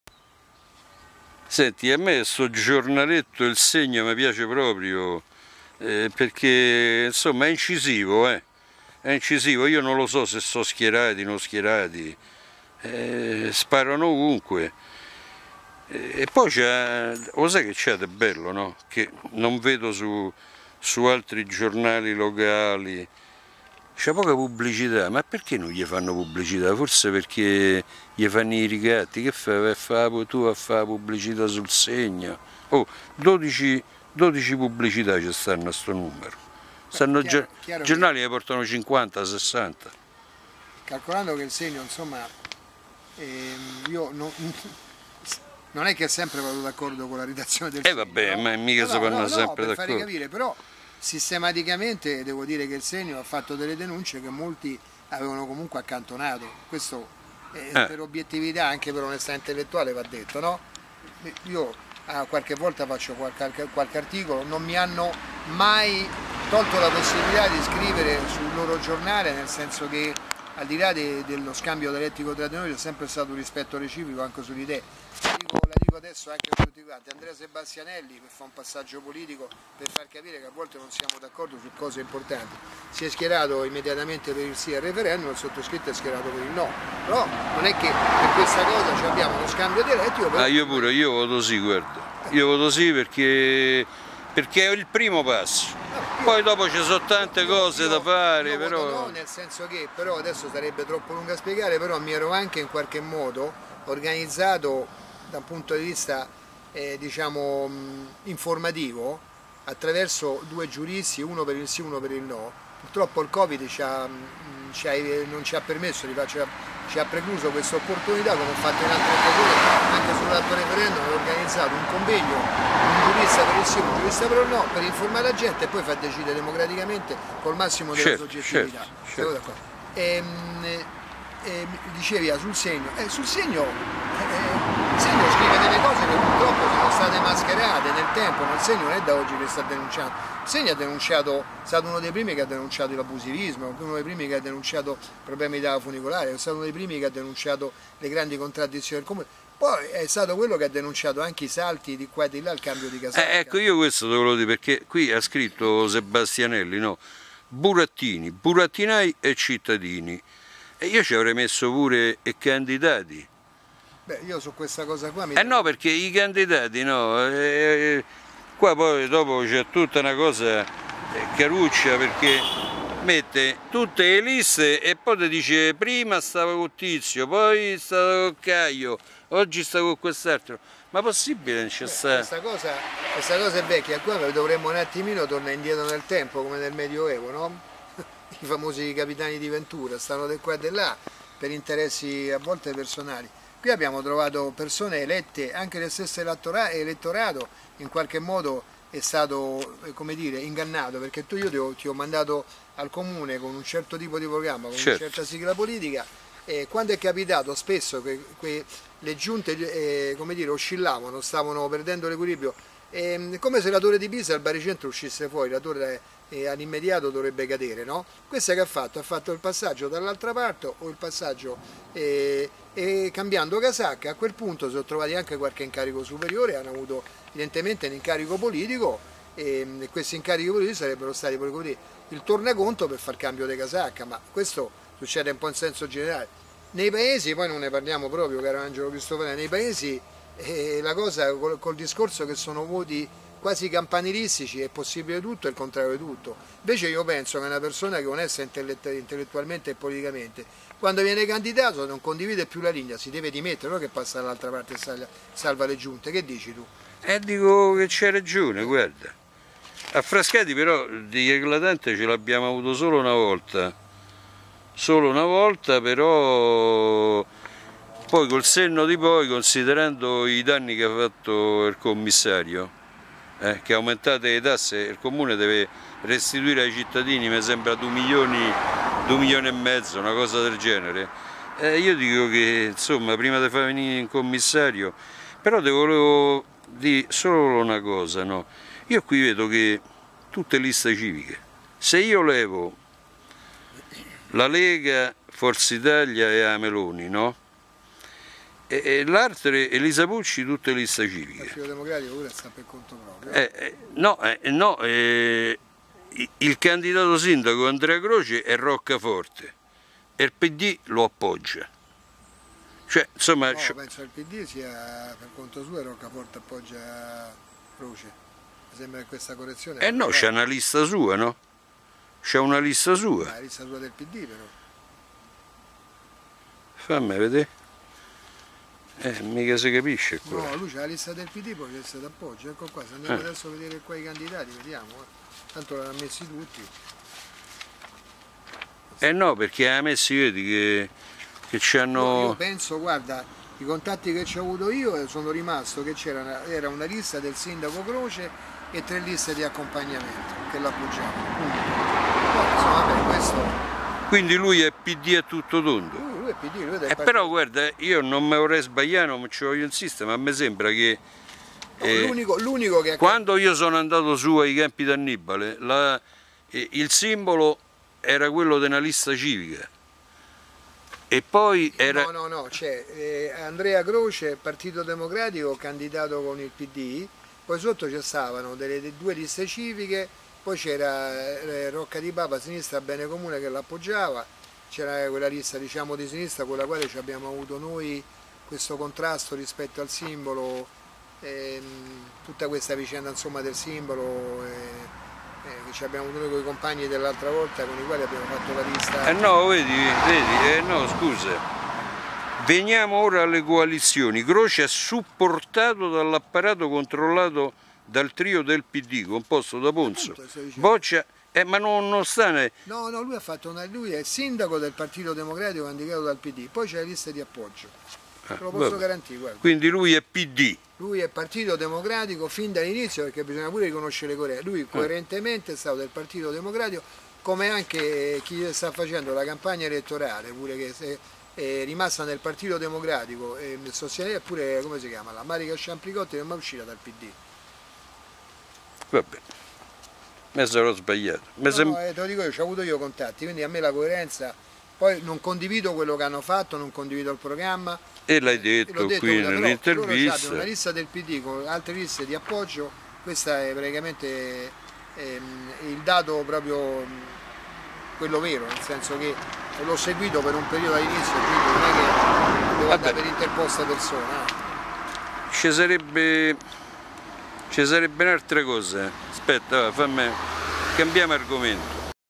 GENNARO SPIGOLA " LO SPIEGONE " - 3 - PARLIAMO de IL SEGNO
12 pubblicità su 24 pagine. Neppure una " immaginetta " sui partecipanti alla tornata elettorale.
LE INCHIESTE TOSTE nel tempo portate avanti, inchieste scomode mai viste sui giornaletti locali !
I SALTI DELLA QUAGLIA, I CAMBI DI CASACCA I TRADIMENTI di chi si fa eleggere su un programma e poi ci ripensa .dajeeeee !